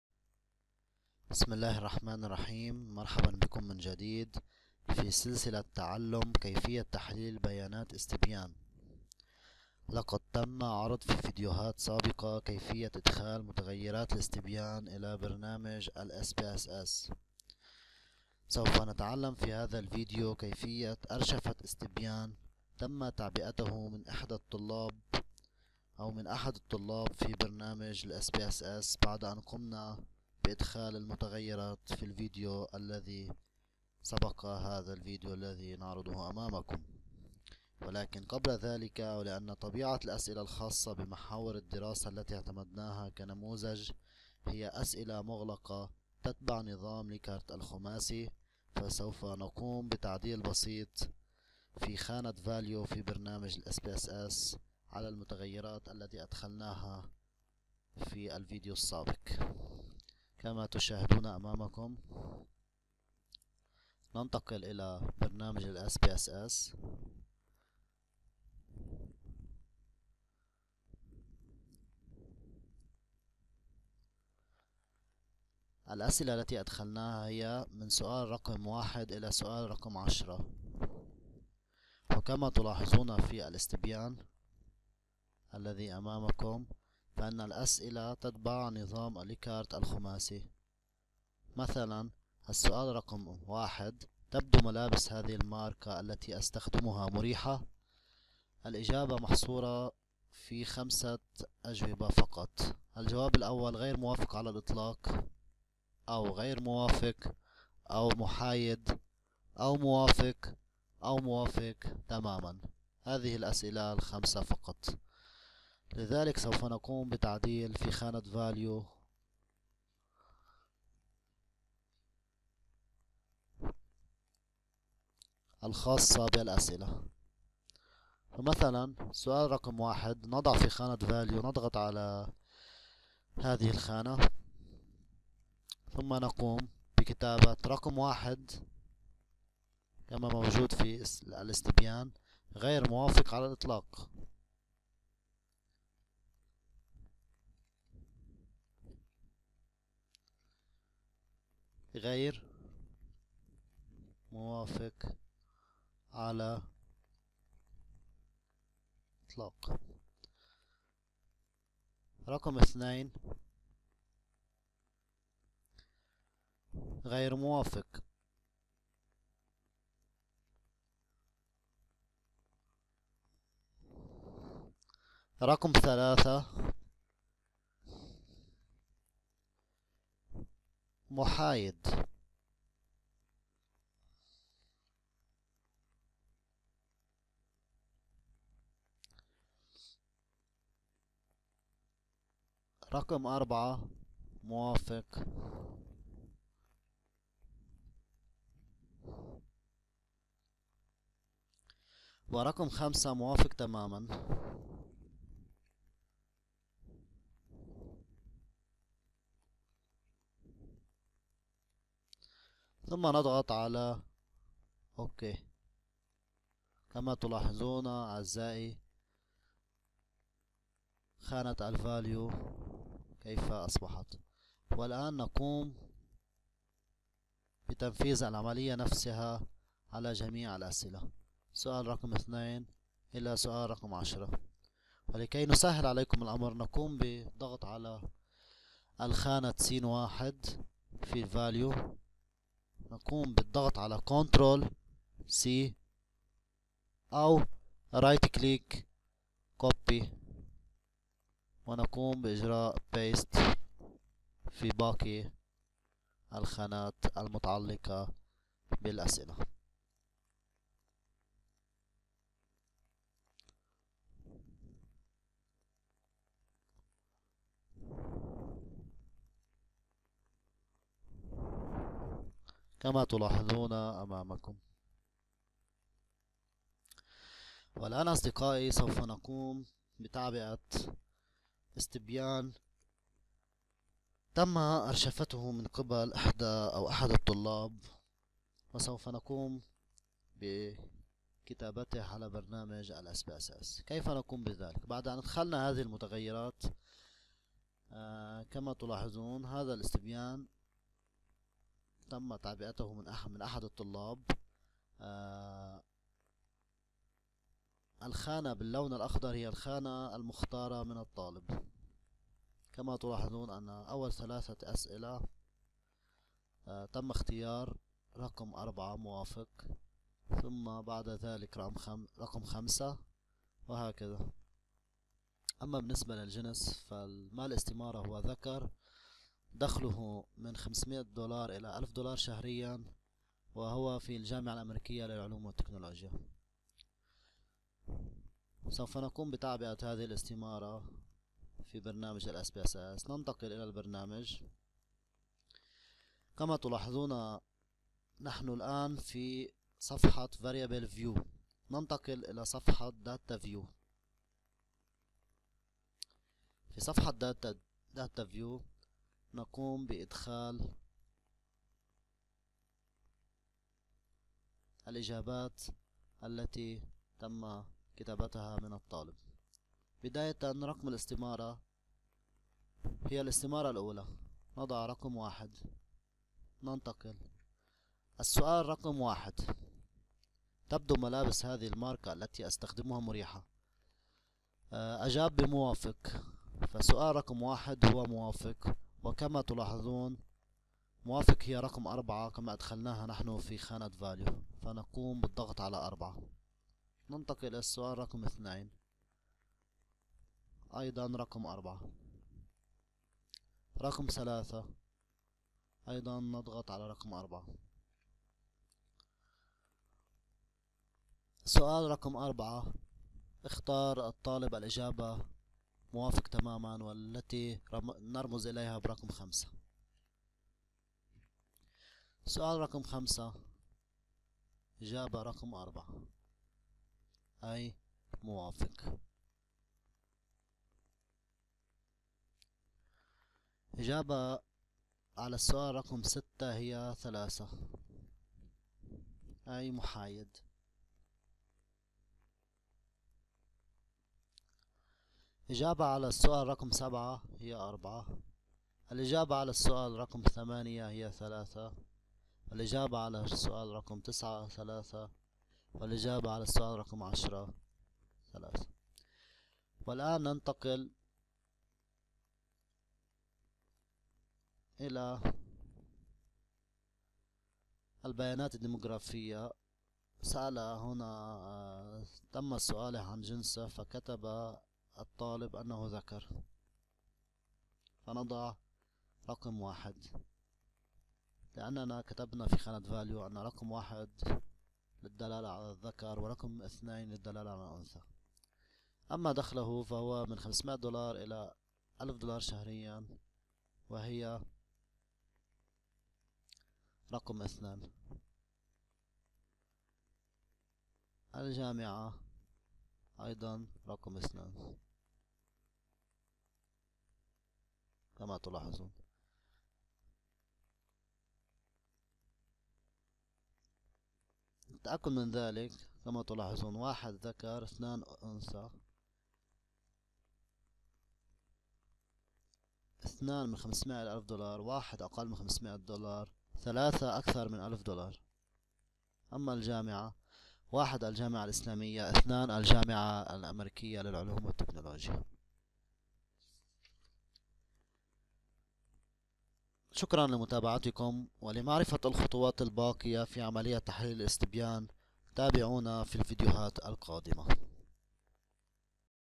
تحليل الاستبيان: الدرس الرابع | كيفية إدخال بيانات الاستبيان إلى برنامج الـ Spss
لمشاهدة الدورة الكاملة لكيفية التحليل الاحصائي للبيانات باستخدام برنامج الــ
                                    SPSS
              نرجو الدخول إلى قناتنا الجديدة على الرابط الآتي

ساعدونا لنصل إلى 100k مشترك
تحليل نموذج استبيان خاص بدراسة ما بشكل كامل حيث سنتعرض من خلال سلسلة فيديوهات كيفية أرشفته على البرنامج وتحليله واختبار الفرضيات وصولاً إلى تحديد نتيجة الدراسة أو البحث، بشكل يمكّن الطلاب من تحليل أبحاثهم التي يقدمونها في رسائل الماجستير أو أي بحث آخر.